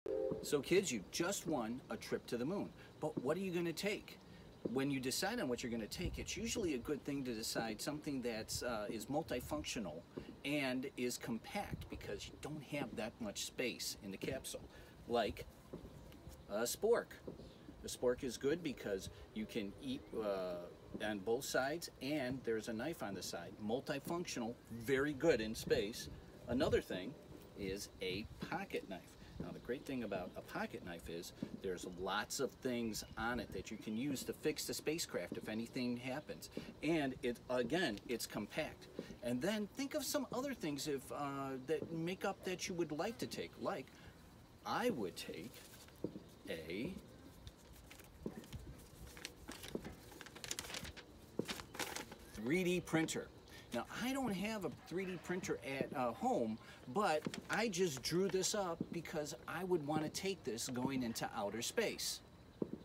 What You Need to Bring to the Moon
There was a great deal of thought that was put into what was needed on a trip to the moon. Compact items with multi-functions are ideal because they don't take up a lot of the minimal space you have to live in on your 3 day trip.
Be creative, use things from around the house. If you don't have them, or it doesn't exist, make it or draw it.